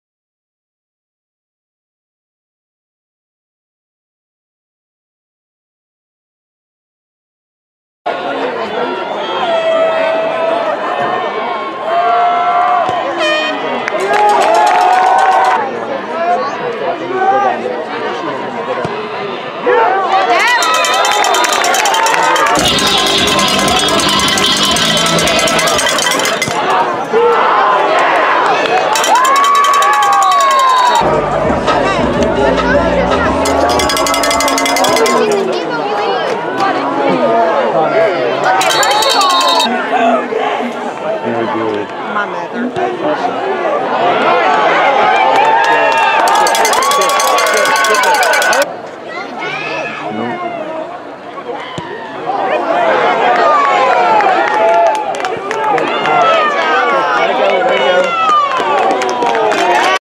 Ewan Johnson's David Crockett Highlights
Ewan Johnson Class of 2021 K/P
2/3 FGs (26, 29, Just short from 52), 3/3 PATs, 6/6 TBs, 42 Yard Punt Avg.
South Doyle HS, Knoxville, TN